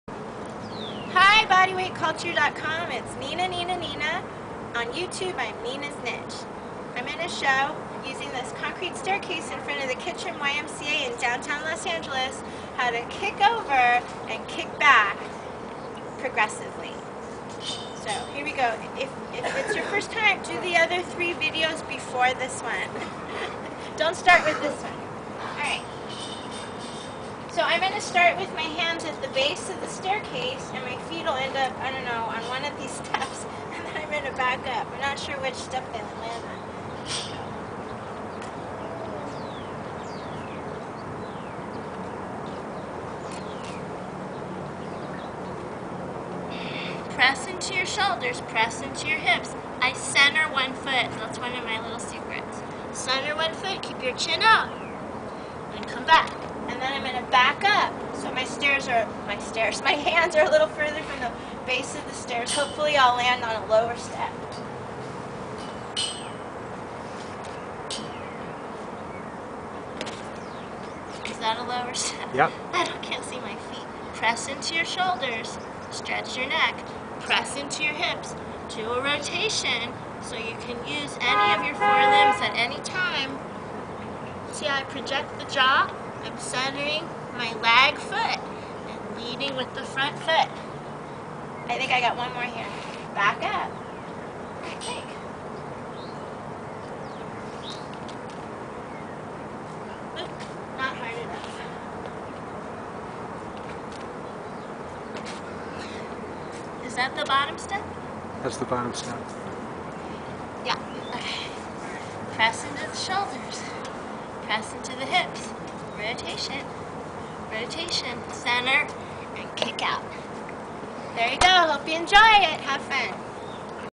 Progressive Front Limbers using a staircase: BodyWeightCulture.com NinaNinaNina
Learn to kick over and back using any staircase for practicing Front Limbers.  This is the 4th of 4 videos.  It is the most challenging of the staircase videos for NinasNiche on YouTube.  View the others first if you are not ready to do this one right away.